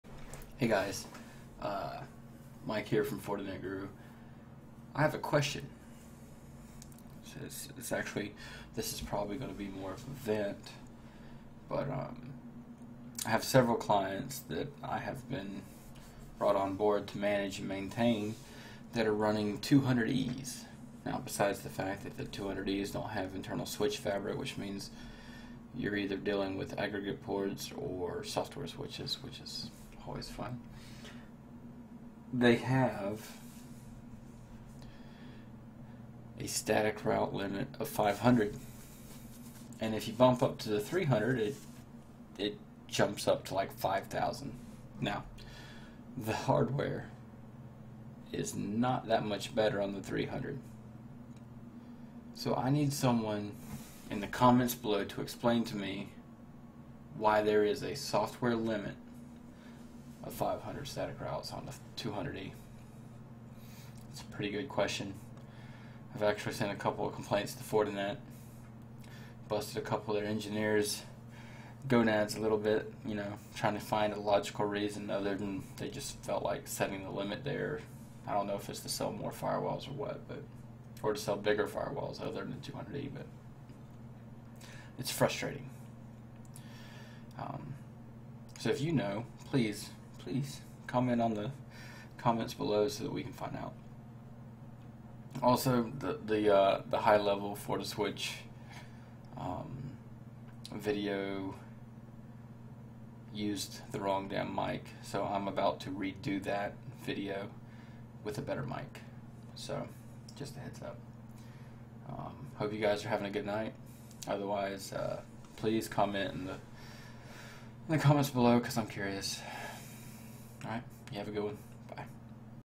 What The Crap Fortinet?!? Static Route Limit Complaints
Why does the 200E have a limit of 500 static routes?!?!?! It has plenty of hardware to support more!